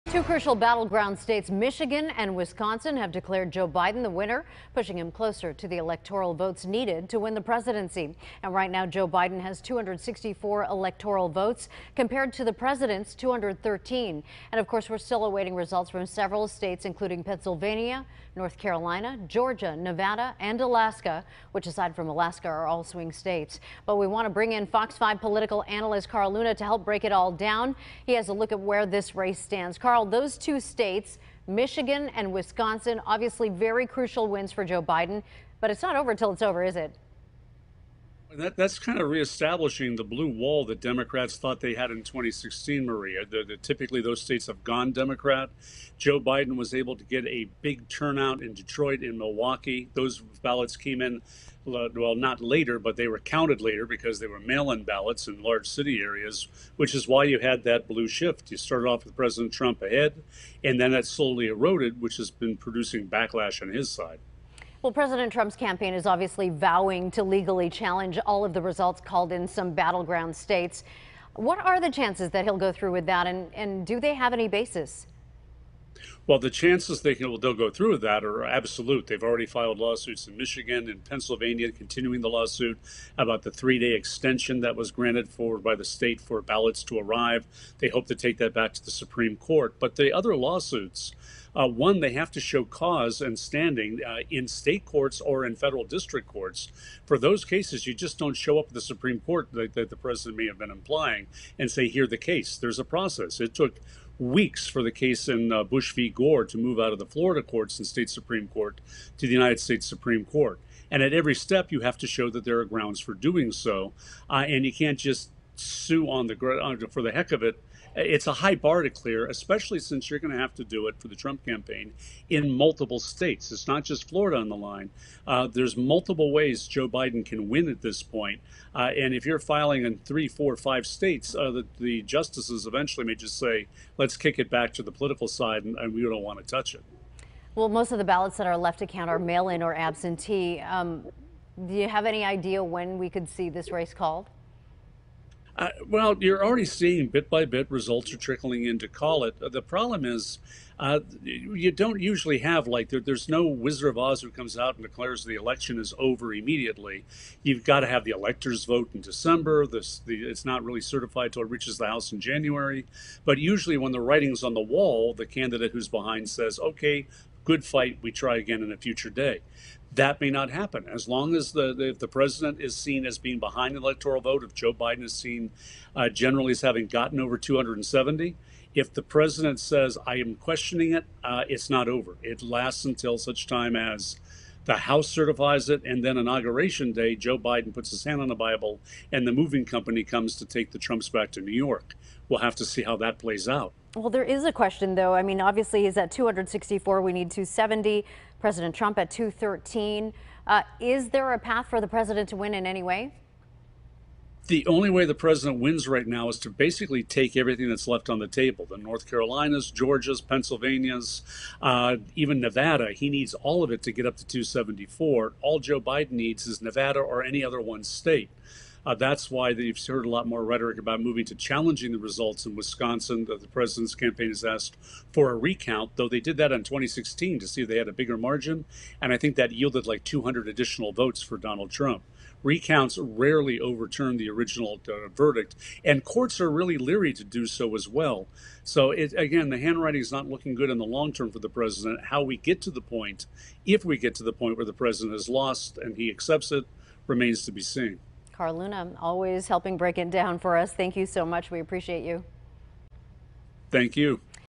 FOX 5 Interview With Carl Luna: Election Day Results
FOX 5 political analyst Carl Luna looks at where the race for president stands as of Wednesday.